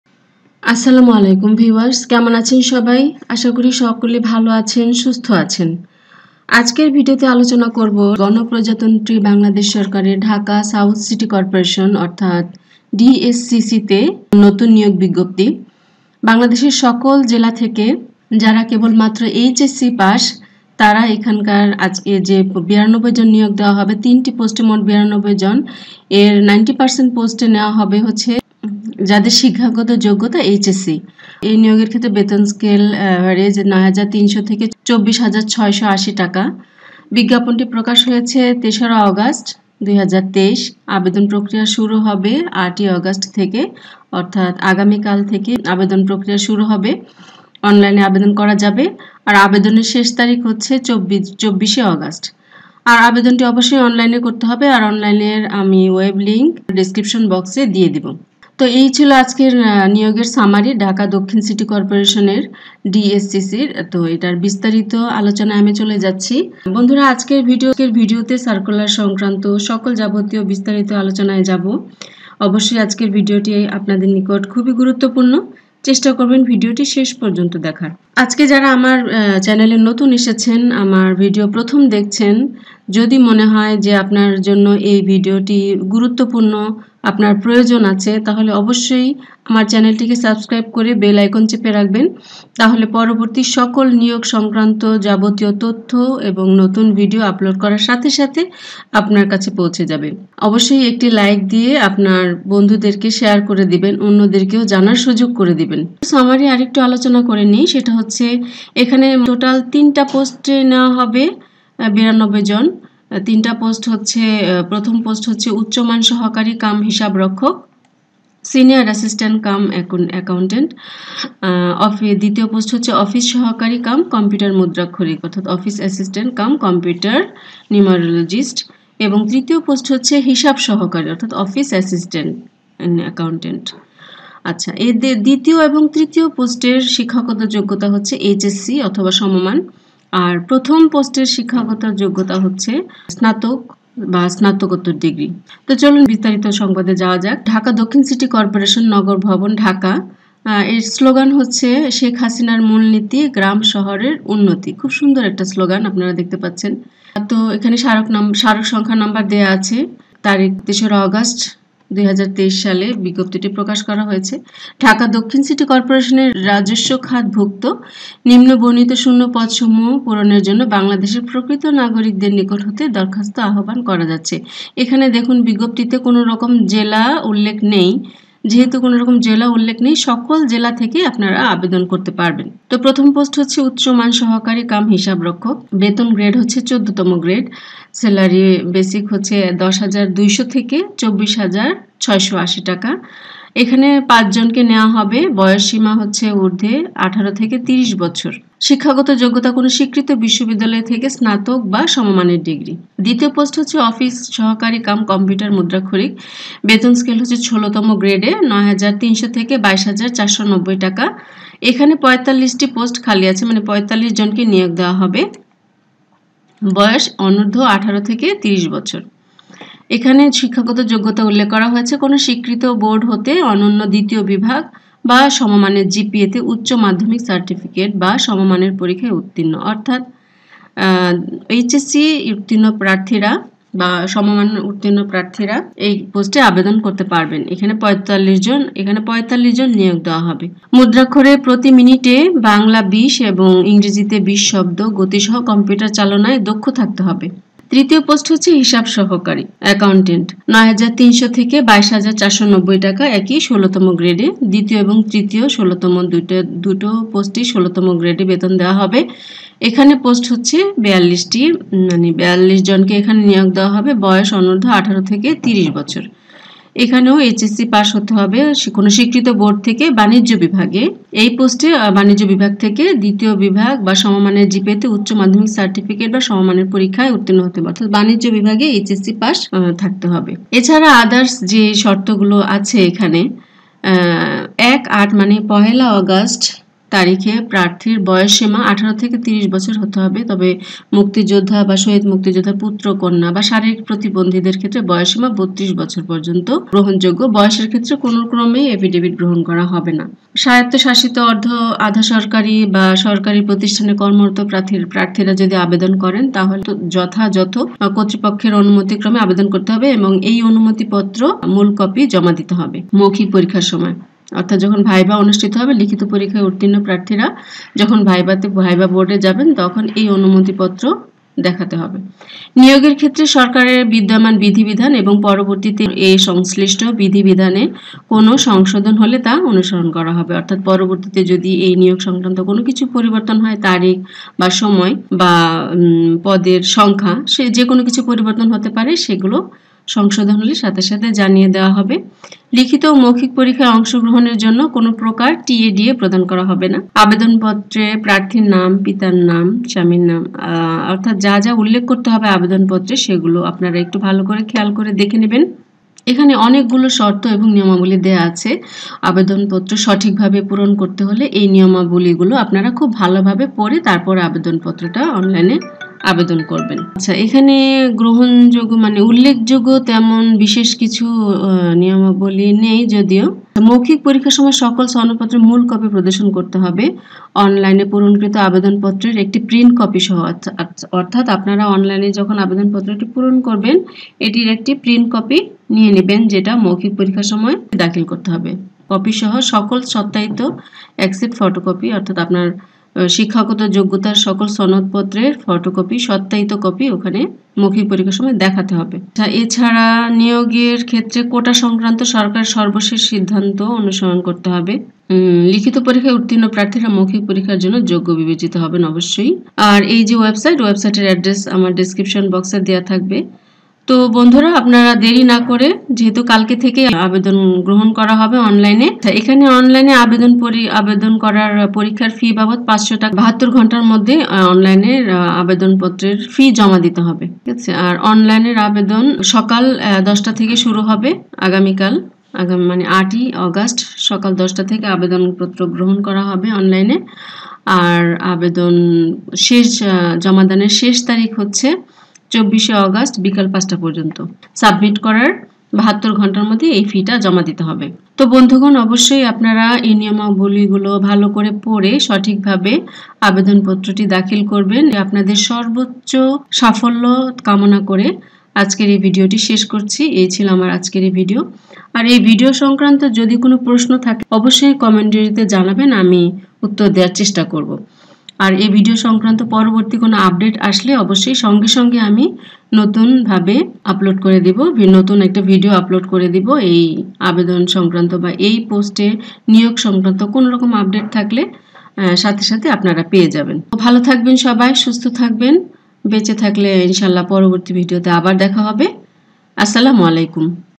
ঢাকা দক্ষিণ সিটি কর্পোরেশনে নিয়োগ (ডিএসসিসি) ২০২৩ || DSCC job circular.
ঢাকা দক্ষিণ সিটি কর্পোরেশনে নিয়োগ (ডিএসসিসি)  || DSCC job circular | নিয়োগ বিজ্ঞাপ্তি 2023

৩ টি পোস্টে মোট ৯২ জন নিয়োগ

বিজ্ঞাপ্তি প্রকাশের তারিখ : ০৩ আগস্ট, ২০২৩
আবেদন প্রক্রিয়া শুরুর হবেঃ ০৮  আগস্ট,,২০২৩
আবেদনের শেষ তারিখ : ২৪  আগস্ট, ২০২৩